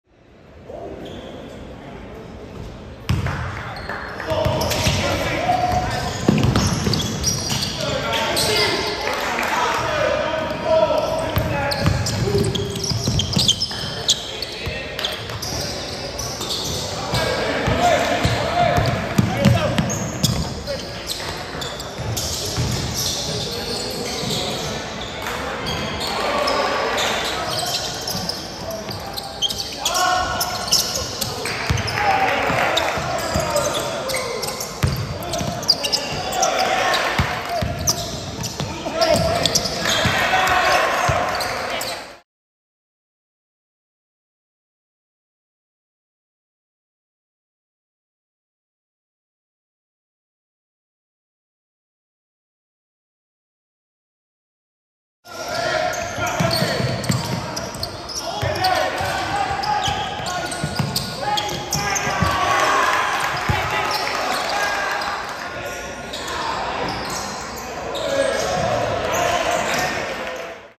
GMC Hoops Highlights - Monsignor Farrell vs. Perth Amboy - Raritan Bay Summer League - July 22, 2024
Watch highlights from the action in Perth Amboy’s 48-23 victory over Monsignor Farrell at the 2024 Raritan Bay Summer League on Monday night in South Amboy.